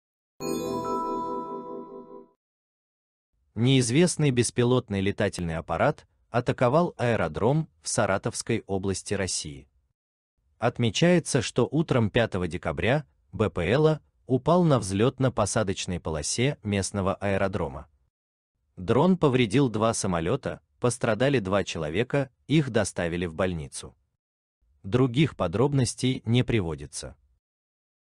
Неизвестный беспилотник атаковал аэродром в Саратовской области
Неизвестный беспилотный летательный аппарат атаковал аэродром в Саратовской области России.

Отмечается, что утром 5 декабря БПЛА упал на взлетно-посадочной полосе местного аэродрома. Дрон повредил два самолета, пострадали два человека, их доставили в больницу.

Других подробностей не приводится.